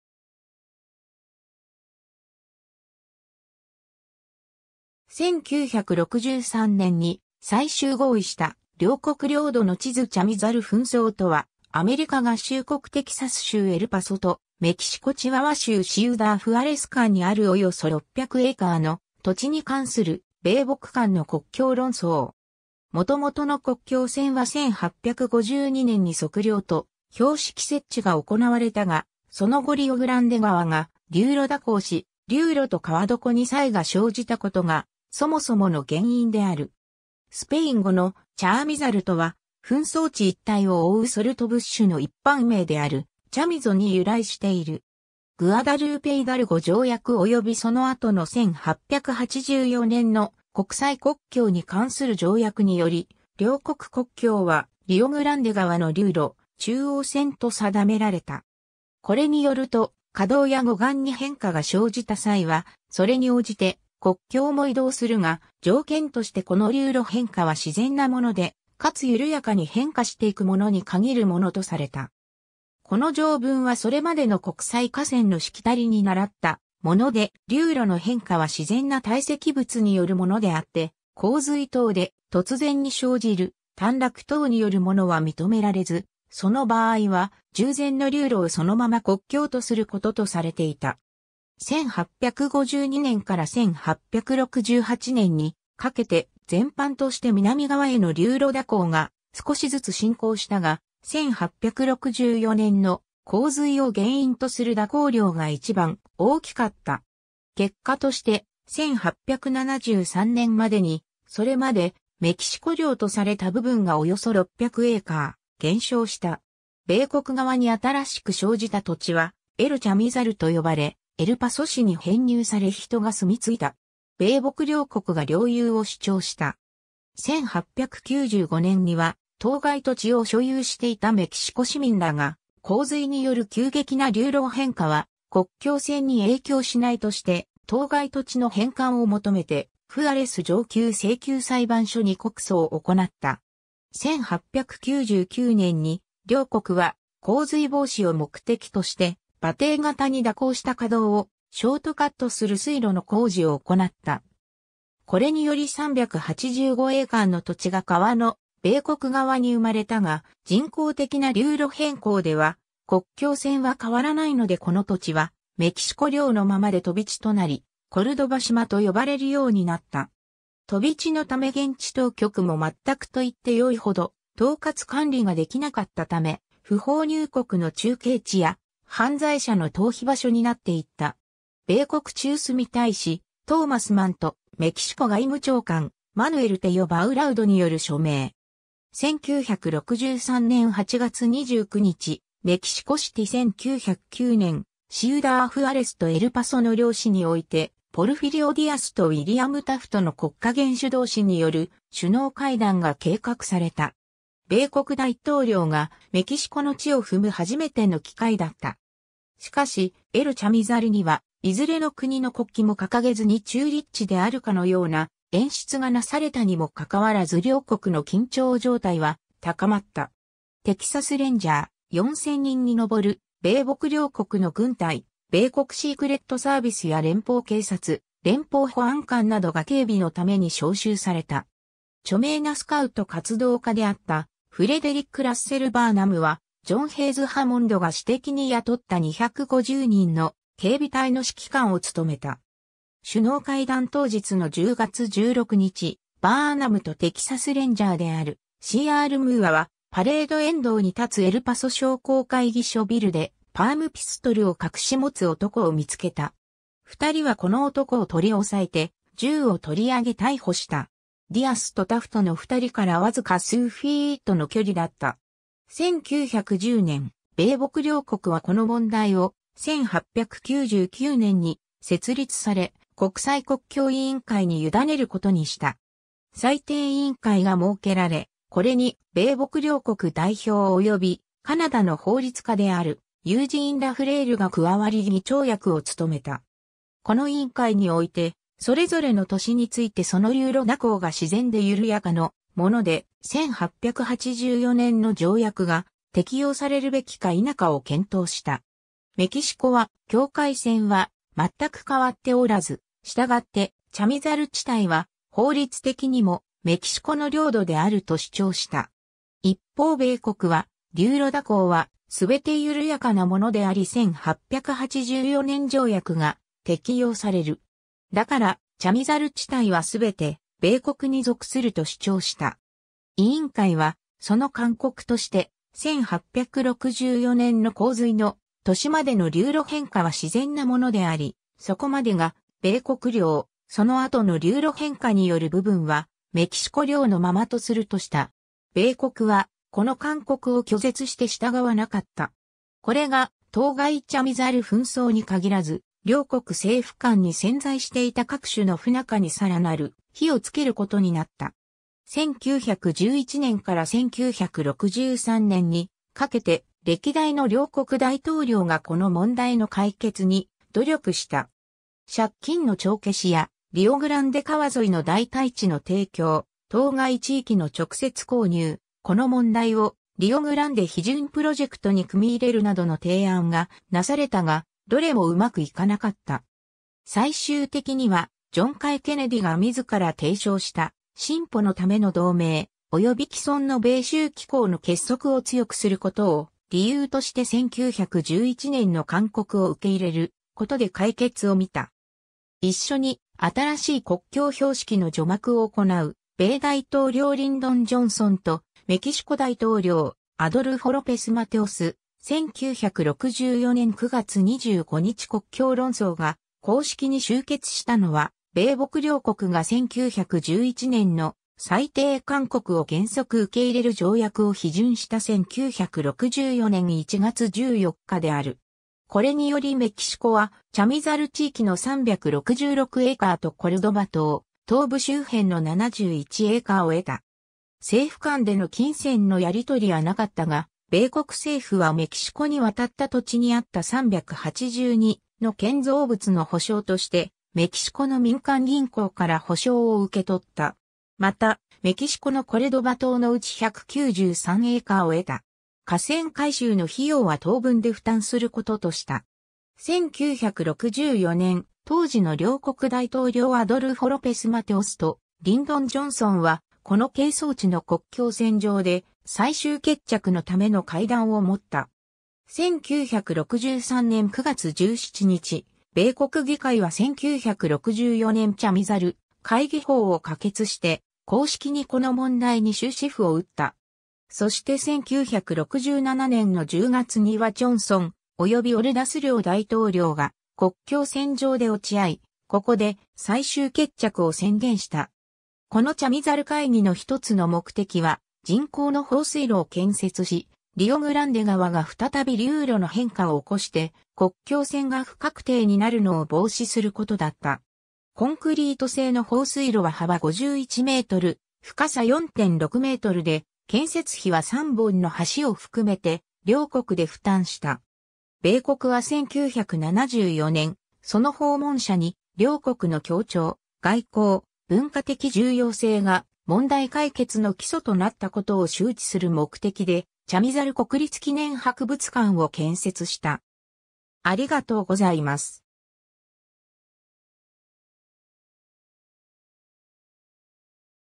チャミザル紛争
アメリカ合衆国の政治史_(1945年-1989年)
アメリカ合衆国・メキシコ関係
アメリカ＝メキシコ国境
エルパソ_(テキサス州)
1963年に最終合意した両国領土の地図 チャミザル紛争（ちゃみざるふんそう）とは、アメリカ合衆国テキサス州エルパソとメキシコ チワワ州シウダー・フアレス間にあるおよそ 600エーカー（2.4平方キロメートル）の土地に関する米墨間の国境論争。
元々の国境線は1852年に測量と標識設置が行われたが、その後リオ・グランデ川が流路蛇行し、流路と河床に差異が生じたことがそもそもの原因である。
 スペイン語の “chamizal”（チャミザル）とは紛争地一帯を覆うソルトブッシュ (Atriplex canescens) の一般名である “chamizo” に由来している。
 グアダルーペ・イダルゴ条約（米墨戦争の講和条約）およびその後の1884年の国際国境に関する条約により、両国国境はリオ・グランデ川の流路中央線と定められた。
これによると、河道や護岸に変化が生じた際は、それに応じて国境も移動するが、条件としてこの流路変化は自然なもので、かつ緩やかに変化していくものに限るものとされた。
この条文はそれまでの国際河川のしきたりに倣ったもので、流路の変化は自然な堆積物によるものであって、洪水等で突然に生じる短絡等によるものは認められず、その場合は、従前の流路をそのまま国境とすることとされていた。
 1852年から1868年にかけて、全般として南側への流路蛇行が少しずつ進行したが、1864年の洪水を原因とする蛇行量が一番大きかった。
結果として、1873年までにそれまでメキシコ領とされた部分がおよそ600エーカー（2.4平方キロメートル）減少した。
米国側に新しく生じた土地は「エル・チャミザル (El Chamizal)」と呼ばれ、エルパソ市（米国）に編入され人が住み着いた。
米墨両国が領有を主張した。
1895年には当該土地を所有していたメキシコ市民らが洪水による急激な流路変化は国境線に影響しないとして、当該土地の返還を求めてフアレス上級請求裁判所に告訴を行った。
 1899年に両国は洪水防止を目的として馬蹄形に蛇行した河道をショートカットする水路の工事を行った。
これにより385エーカーの土地が川の米国側に生まれたが、人工的な流路変更では国境線は変わらないのでこの土地はメキシコ領のままで飛び地となり、コルドバ島と呼ばれるようになった。
飛び地のため現地当局も全くといってよいほど統轄管理ができなかったため、不法入国の中継地や犯罪者の逃避場所になっていった。
 米国駐墨大使トーマス・マン（Thomas C Mann、左）とメキシコ外務長官マヌエル・テヨ・バウラウド（Manuel Tello Baurraud、右）による署名。
1963年8月29日、メキシコシティ 1909年、シウダー・フアレスとエルパソの両市においてポルフィリオ・ディアス（メキシコ大統領）とウィリアム・タフト（米国大統領）の国家元首同士による首脳会談が計画された。
米国大統領がメキシコの地を踏む初めての機会だった。
しかし、エル・チャミザルにはいずれの国の国旗も掲げずに中立地であるかのような演出がなされたにもかかわらず両国の緊張状態は高まった。
テキサス・レンジャー、4,000人に上る米墨両国の軍隊、米国シークレットサービスや連邦警察、連邦保安官などが警備のために召集された。
著名なスカウト活動家であったフレデリック・ラッセル・バーナム (Frederick Russel Burnham) は、ジョン・ヘイズ・ハモンド（タフトとはエール大学時代からの親友で、1908年の大統領選挙ではタフトの副大統領候補の一人となった。またメキシコに巨額の投資をしていた）が私的に雇った250人の警備隊の指揮官を務めた。
首脳会談当日の10月16日、バーナムとテキサス・レンジャーである C.R.ムーア (Moore) はパレード沿道に建つエルパソ商工会議所ビルでパームピストルを隠し持つ男を見つけた。
二人はこの男を取り押さえて銃を取り上げ逮捕した。
ディアスとタフトの二人からわずか数フィートの距離だった。
 1910年、米墨両国はこの問題を、1899年に設立され国際国境委員会 (International Boundary Commission) に委ねることにした。
裁定委員会が設けられ、こ...